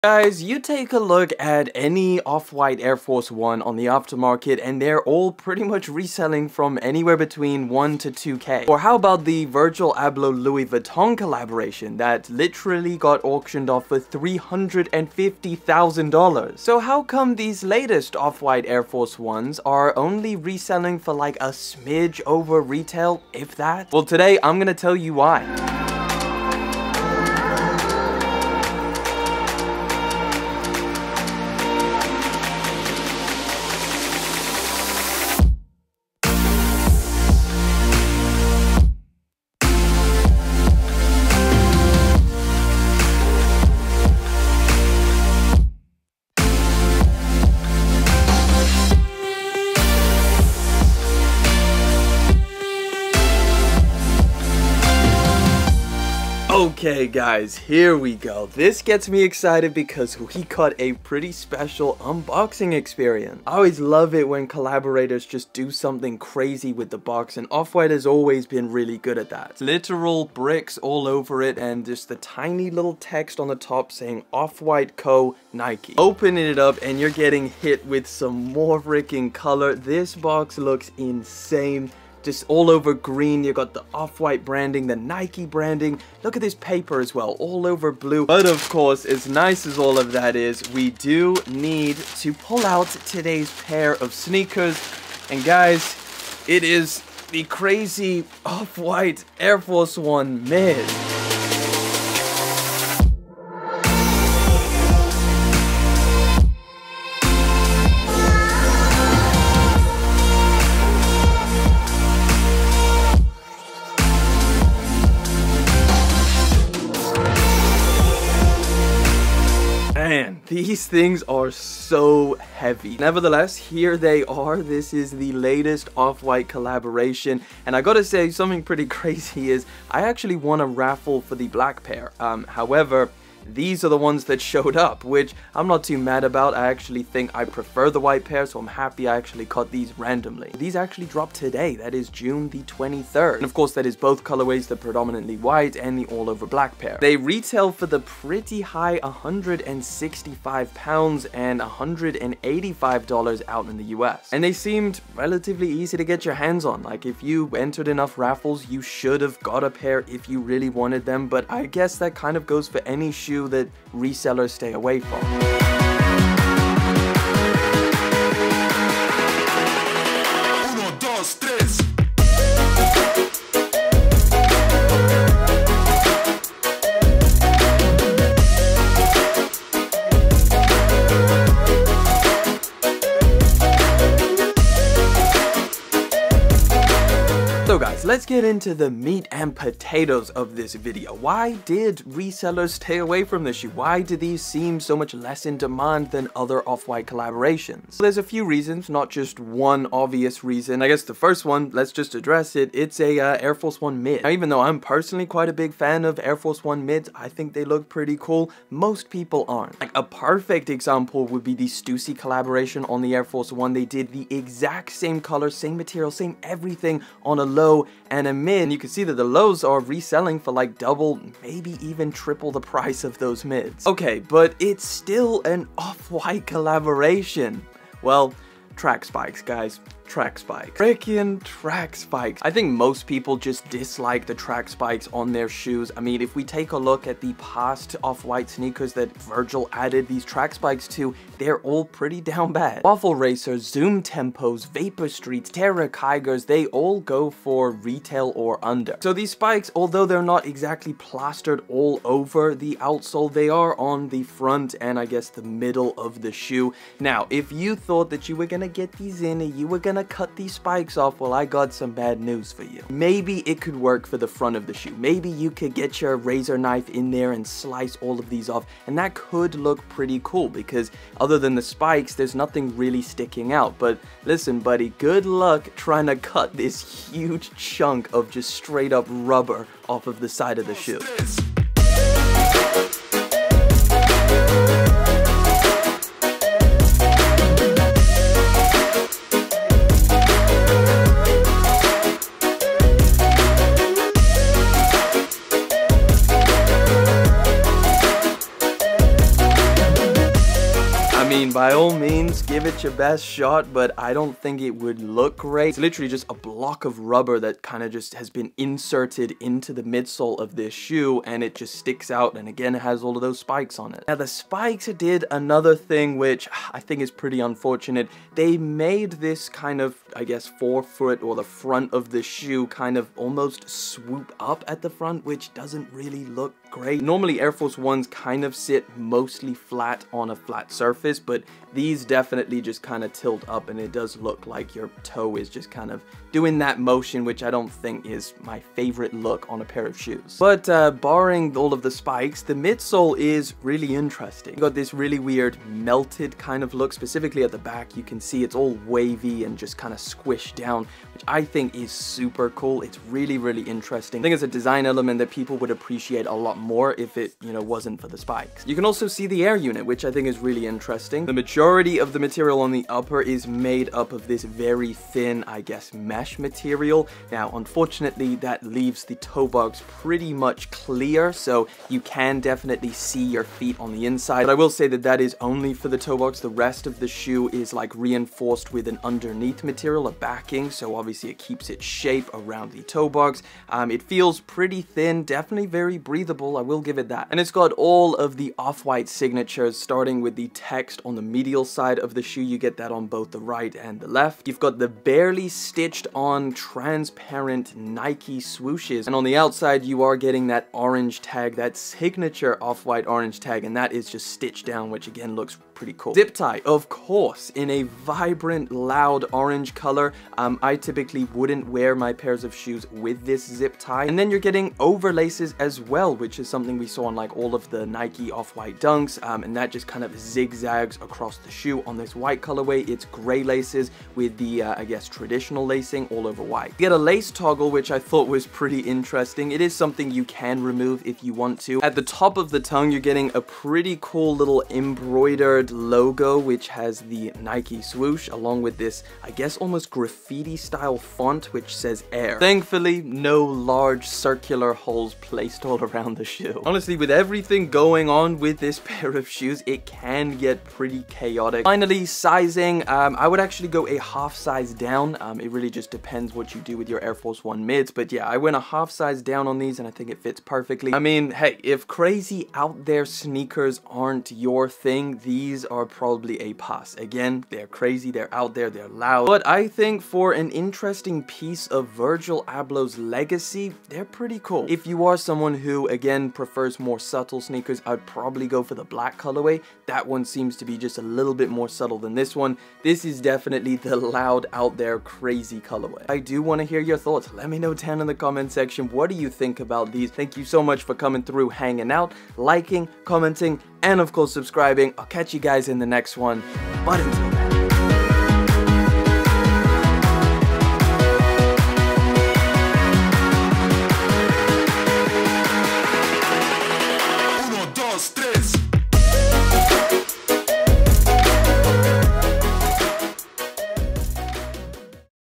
Why There Is No Resell..Off White Air Force 1 Mid Review & On Foot
Today im taking a look at the brand new Off White x Nike Air Force 1 Mid in the White colourway. These dropped June 23rd alongside the Black colourway & retail is set at £165/$185. These are pretty crazy, theres a lot going on with some very interesting design elements and colours. Theres a whole lot to talk about it so be sure to catch the full review!

Let me know what you think of these down in the comments below!